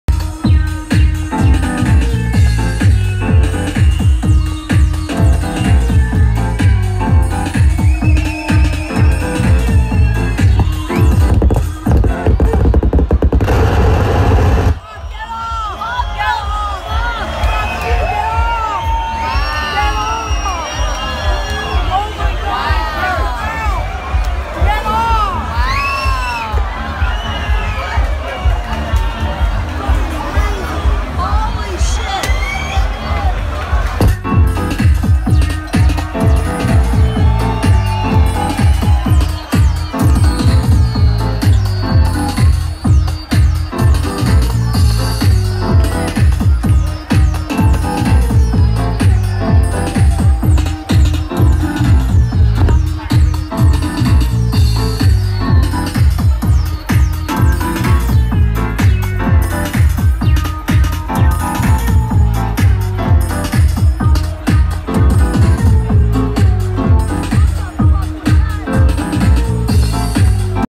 Zip at Sunwaves 21 interrupted by crazy party goer
Girl climbs over the railing and disrupts Zip mid-set, kicking over equipment and cutting out the sound. Donewaves.